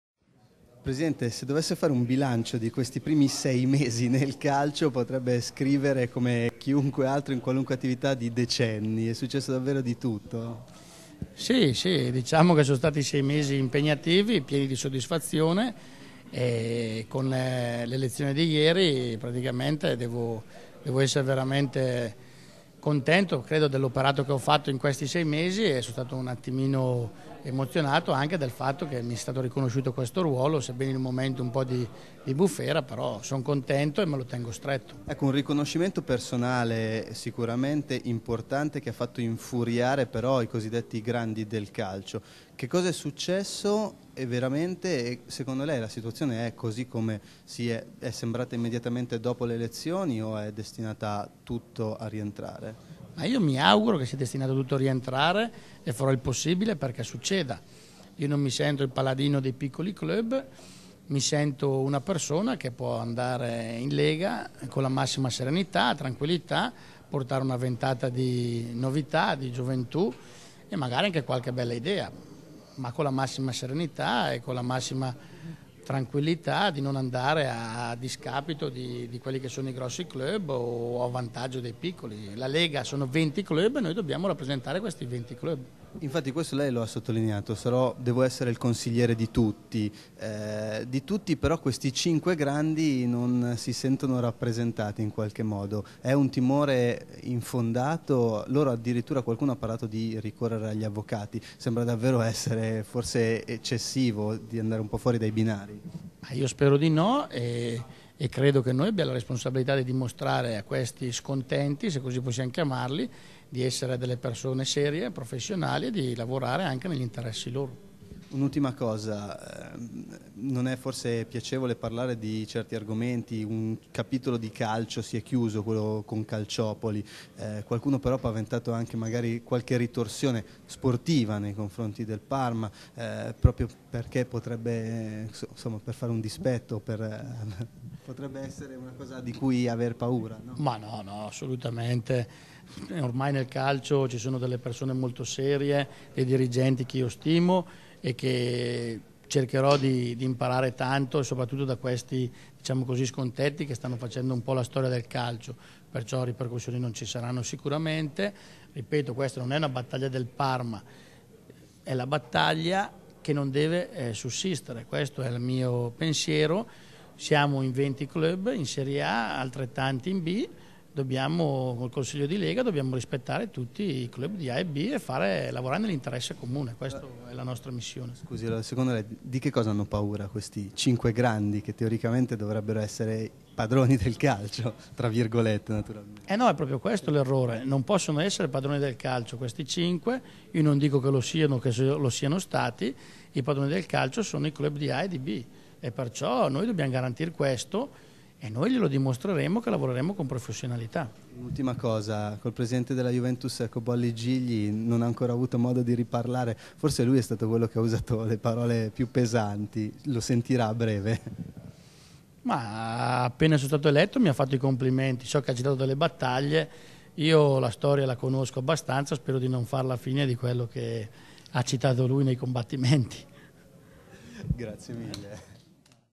tommaso ghirardi 5 settembre 2007
intervista a tommaso ghirardi presidente del parma calcio a collecchio (parma)